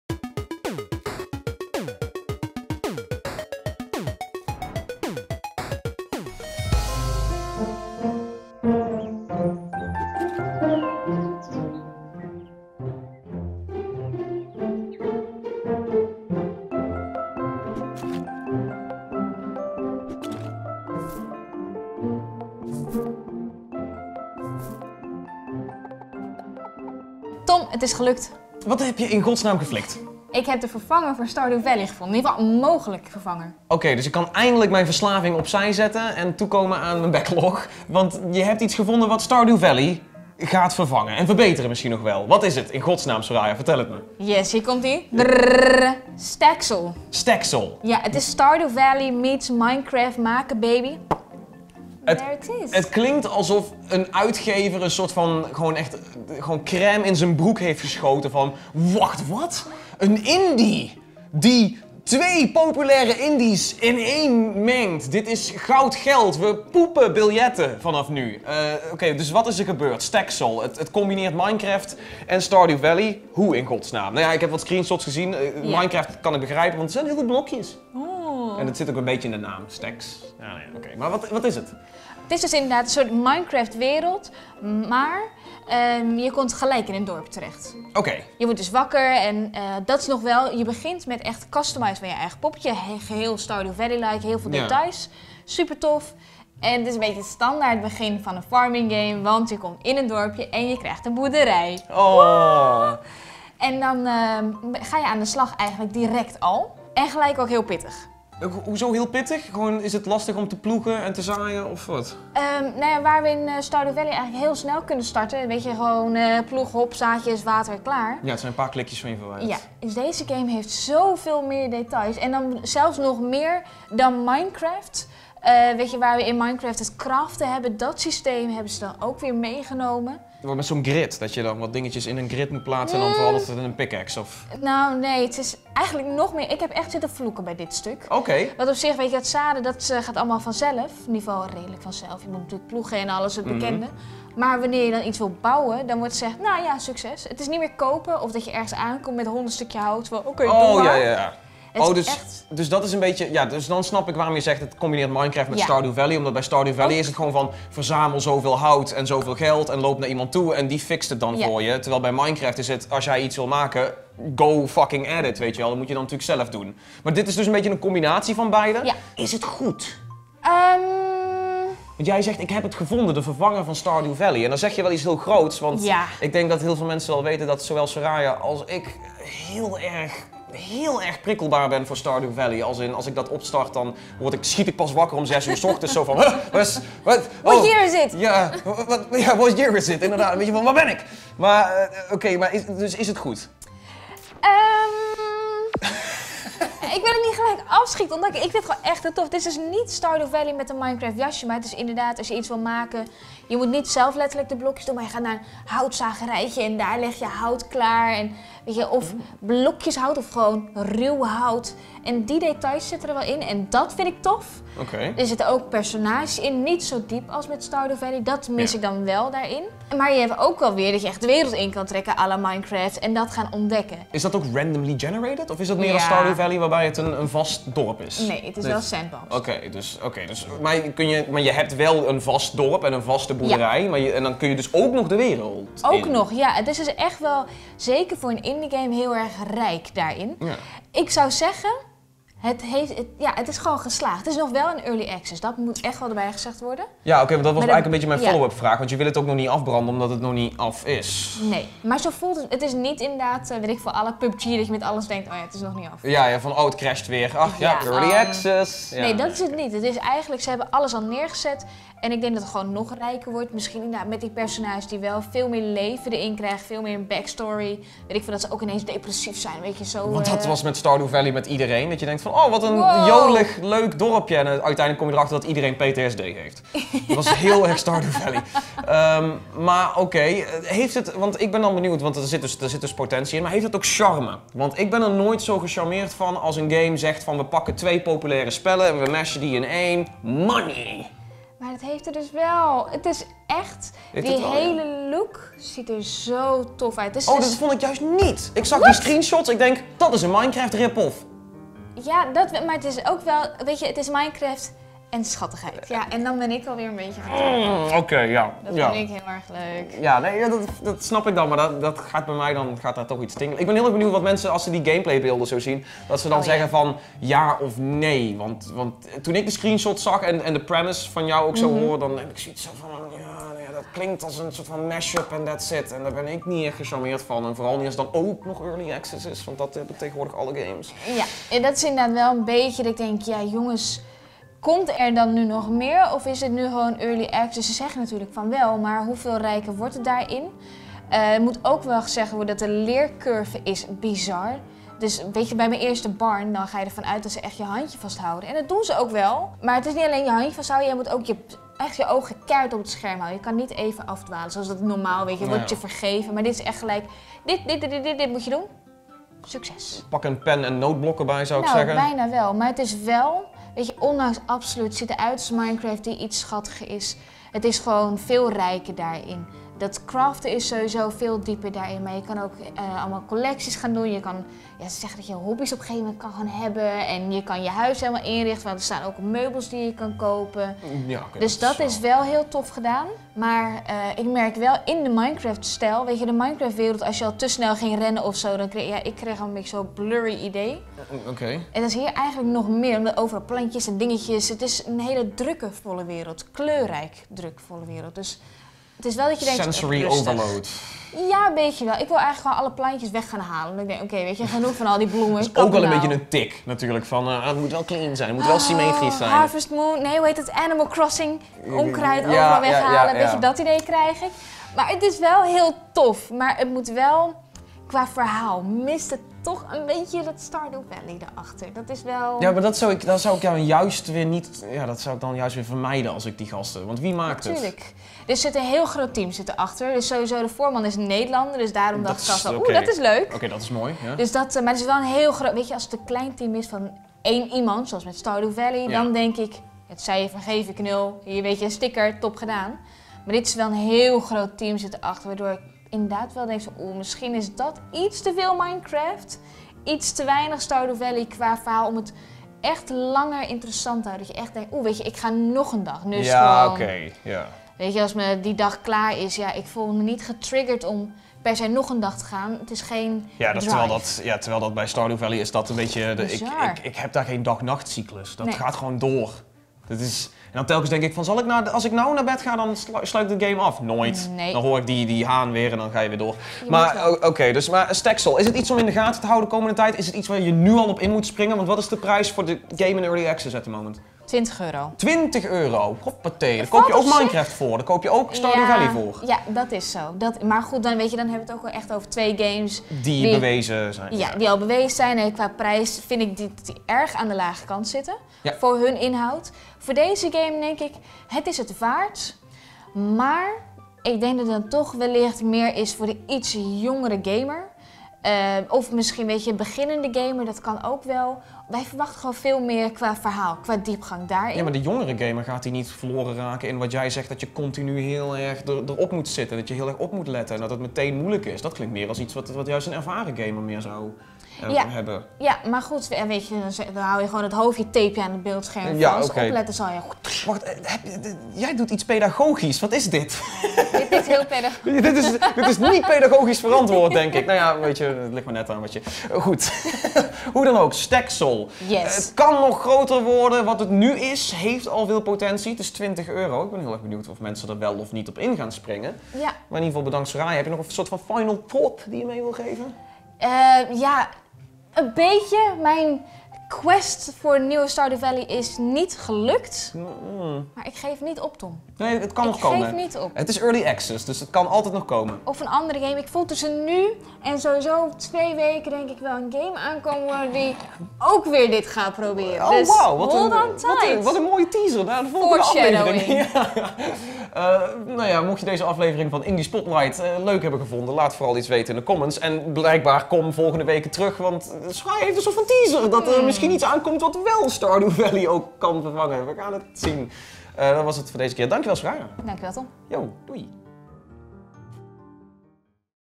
Is Staxel dé samensmelting van Minecraft en Stardew Valley? | Indie Spotlight
Op zoek naar de opvolger van bijvoorbeeld een Minecraft of Stardew Valley? Dan ben je bij Staxel misschien wel aan het juiste adres. Onze indie-dame Soraya is er uitgebreid mee aan de slag geweest en vertelt tegenover Tom wat er zo sterk (en juist niet) aan  dit koddige spelletje is. Dat alles zie je uiteraard in een verse Indie Spotlight.